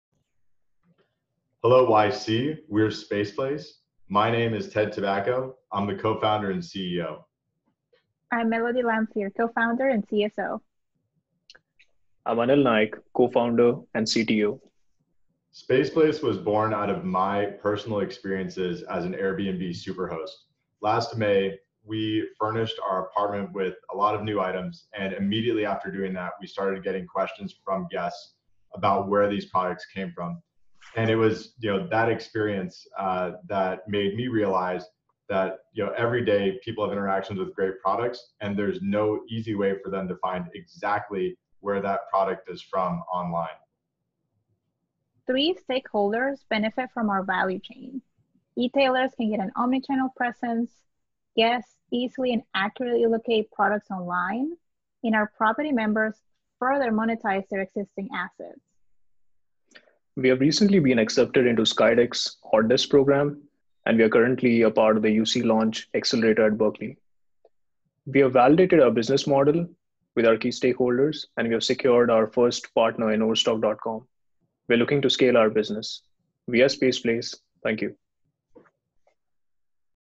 SpacePlace Inc. - Introduction Video - YC
SpacePlace reates organic retail opportunities from physical spaces